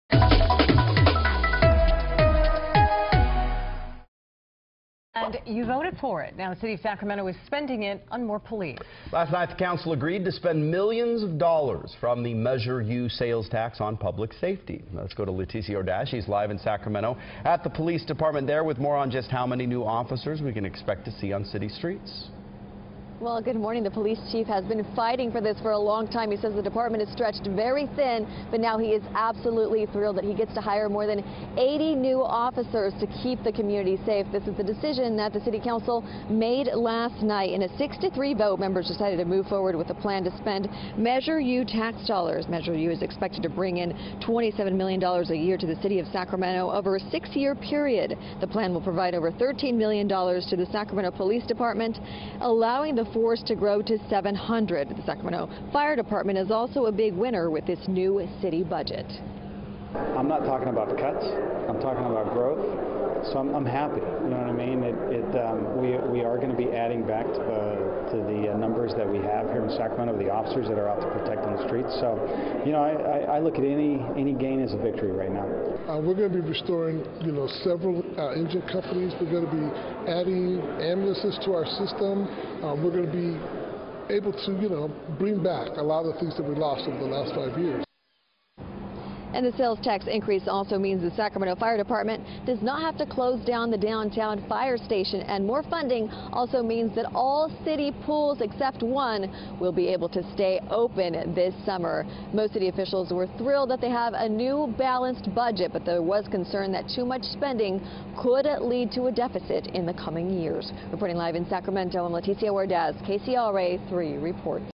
Sac City Council votes to move ahead with Measure U spending
Measure U is expected to bring in $27 million a year to the City of Sacramento over a six-year period.